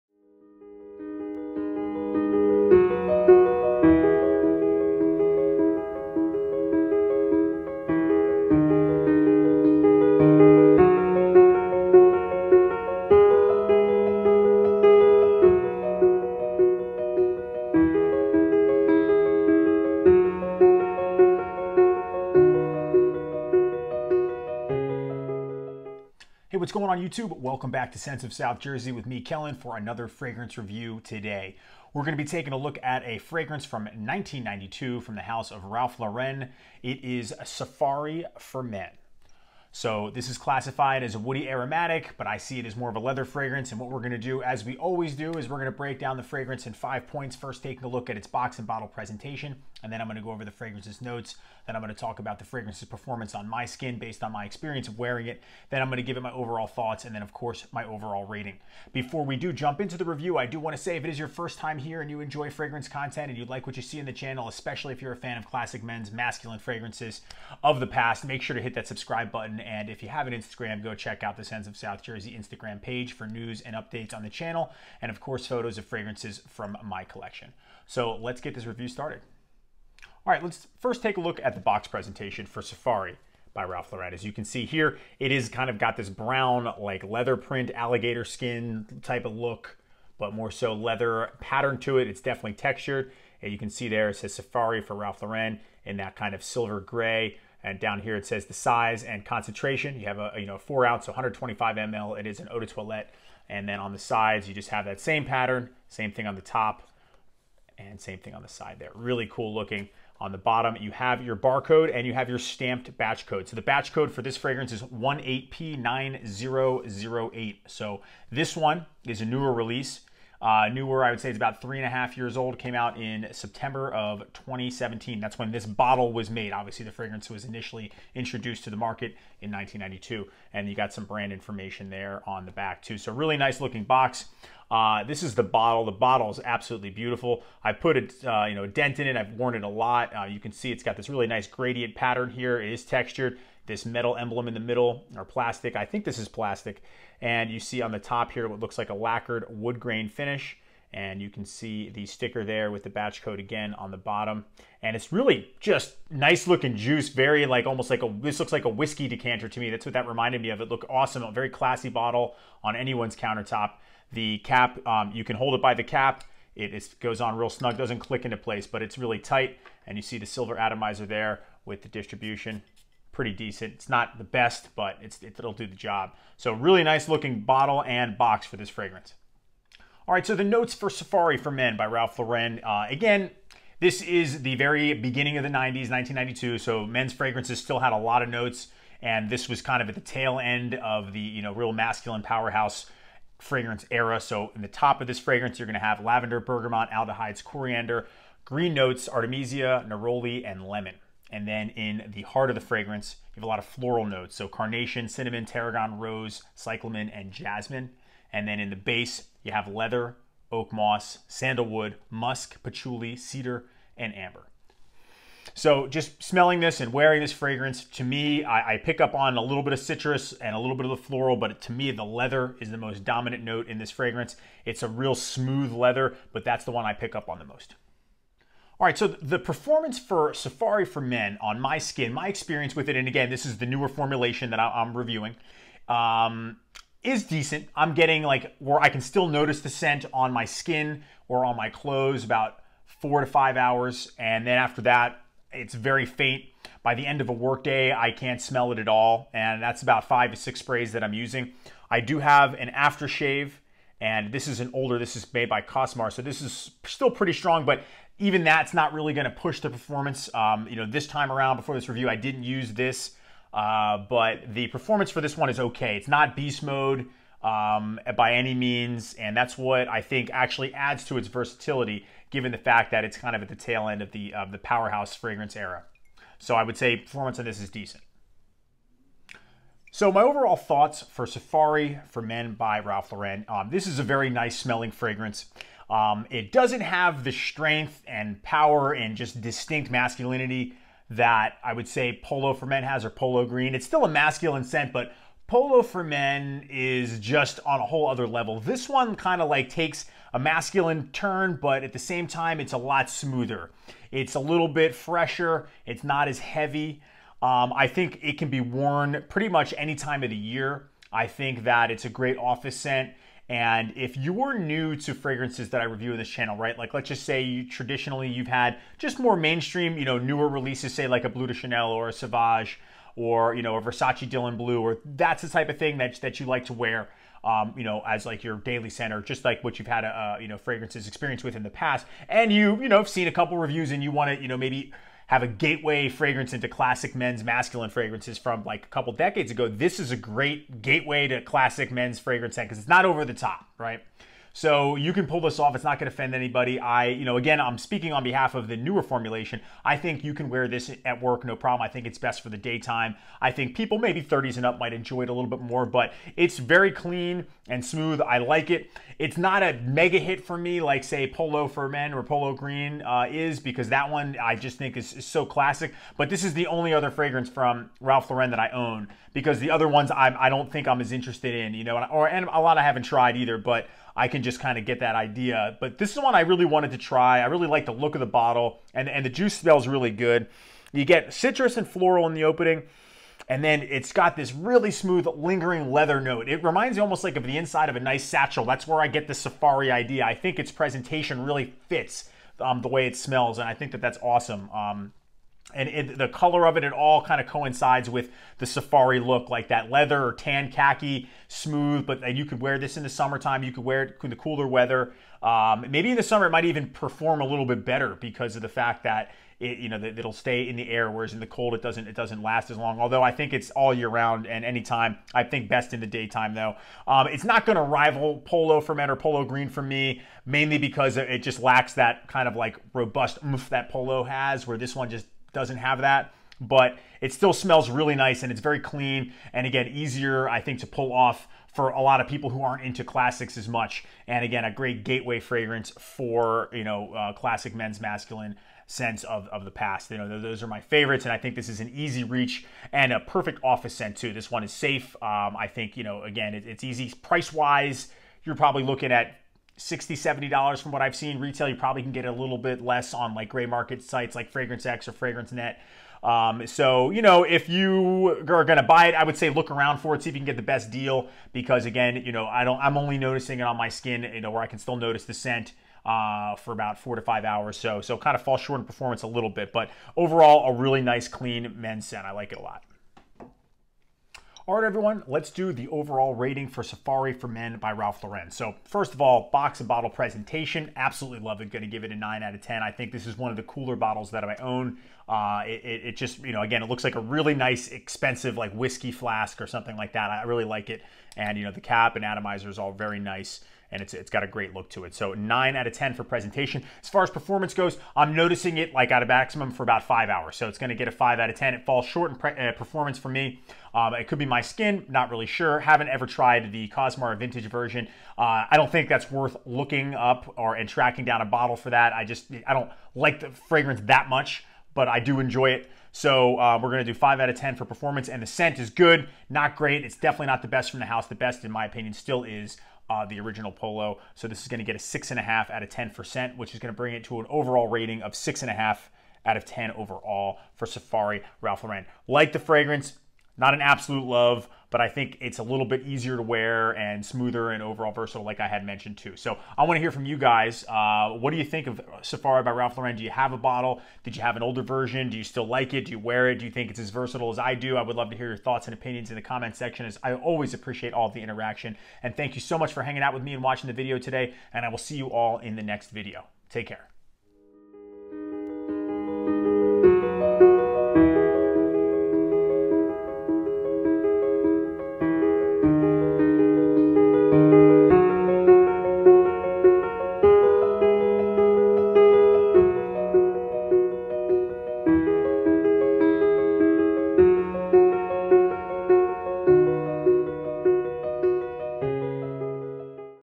Safari for Men by Ralph Lauren Fragrance Review
Safari for Men by Ralph Lauren is a smooth, woody, leather, masculine scent. I feel it’s a versatile and safe fragrance that can be worn year round.

0:00 Intro
1:20 Presentation
3:00 Notes
3:48 Performance
4:39 Overall Thoughts
7:54 Rating
9:00 Closing

Official Instagram: @scentsofsouthjersey